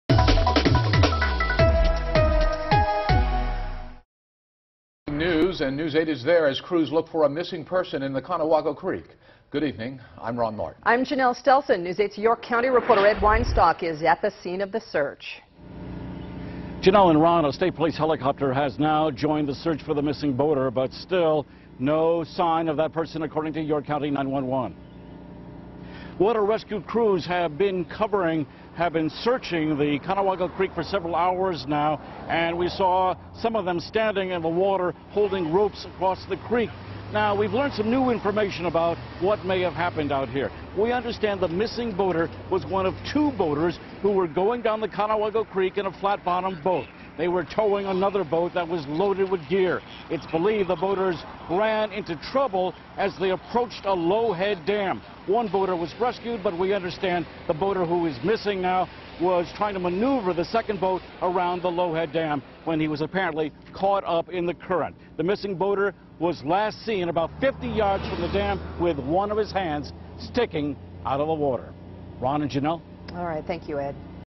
Rescue team searches for missing person in Conewago Creek
A rescue team is searching for a missing person in the Conewago Creek in Washington Township, York County.